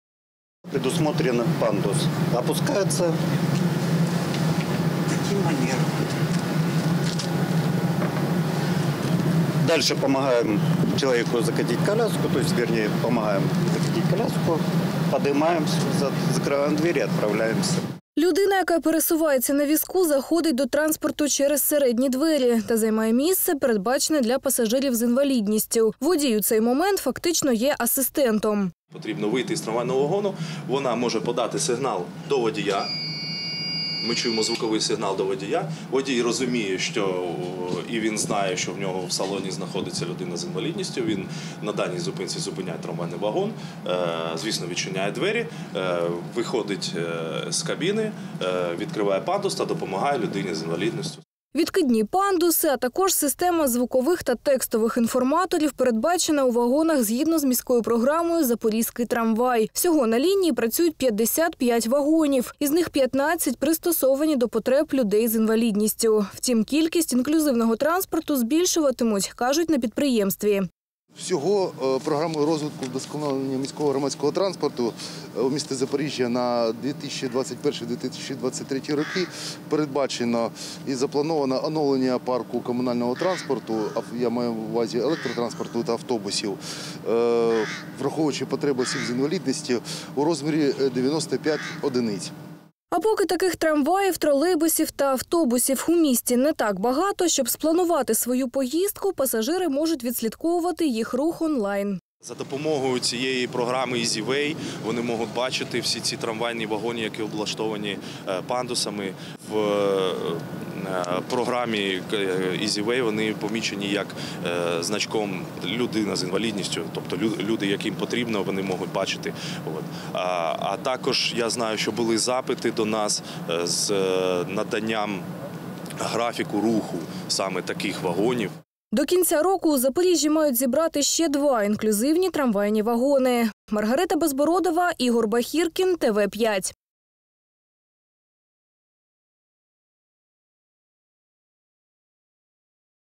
У Запоріжжі презентували оновлений міський транспорт: в чому його особливість
Громадський транспорт доступний для всіх – представники «Запоріжелектротрансу» представили трамвайні вагони, оновлені відповідно до потреб людей з інвалідністю. Скільки їх у Запоріжжі та які ще зміни очікують на міський транспорт дізнавалися наші кореспонденти.